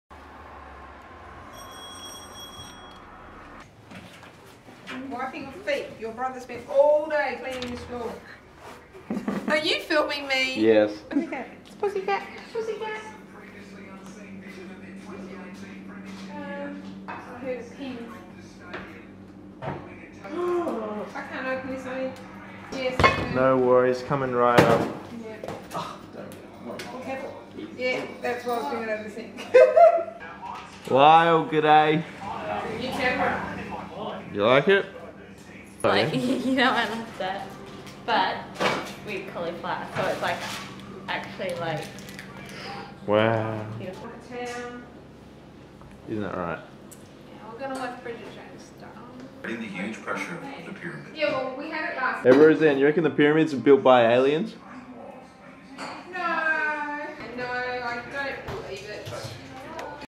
01:05
I got a new camera and I want to practice using it, hopefully by vlogging my family every night my camera skills will progress.